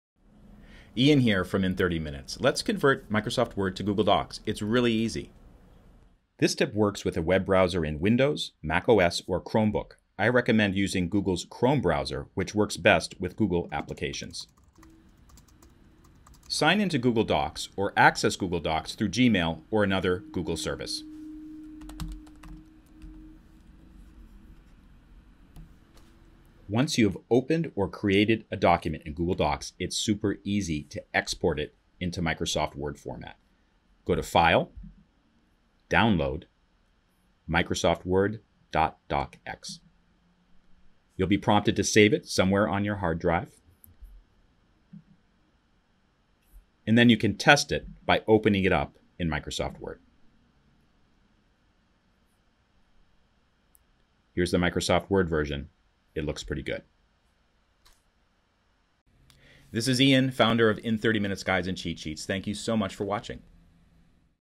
The EASY WAY to convert Google Docs to Microsoft Word (Windows/Mac/Chromebook)
From @in30minutes, learn how to create a Microsoft Word .docx file from Google Docs. Narrator Ian Lamont is the founder of i30 Media Corporation, publisher of IN 30 MINUTES cheat sheets & guides.